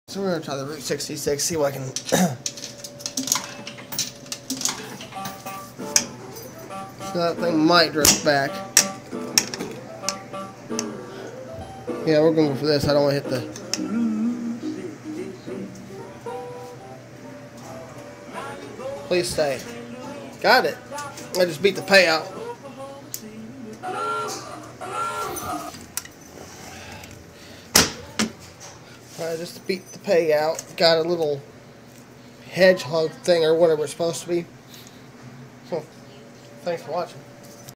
ROUTE 66 CLAW MACHINE@ PIZZA HUT I JUST BEAT PAYOUT LIKE I ALWAYS DO
TODAY I HEAD OVER TO MY LOCAL PIZZA HUT ONLY TO FIND A LOW MACHINE BUT I PLAY THE IT ANYWAY WITH THE WAY THIS MACHINE IS SET WHEN IT GETS LOW ON PLUSH THEN IT BECOMES HARDER TO WIN BECAUSE THE CLAW RARELY HANGS ON TO ANYTHING ANYTIME I PLAYED IT  SO I HOPE YOU ENJOY THIS VIDEO